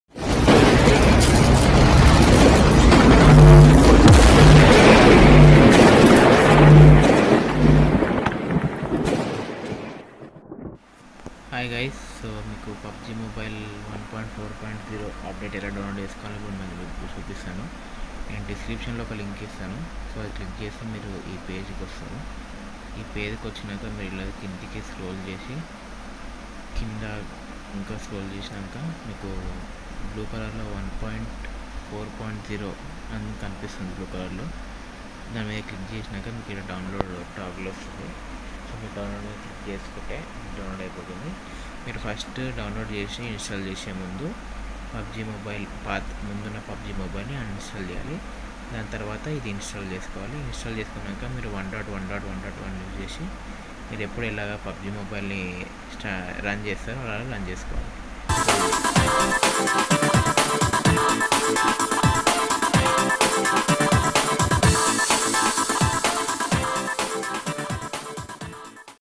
HOW TO DOWNLOAD PUBG MOBILE NEW 1.4.0 UPDATE IN TELUGU
DOWNLOAD 1.1.1.1 FROM PLAYSTORE
AND UNINSTALL THE PREVIOUS PUBG BEFORE INSTALLING THIS UPDATE